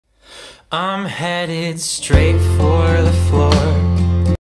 im heading straight for the floor
i dont know what i was doing for this lol

Sorry I haven’t posted in a while

this is a new character that i made! she’s a Chinese dragon and i really love how i made her design :)))

also if you have a name for this character tell me cuz idk what to name her

also I’ll post the link to the song in a second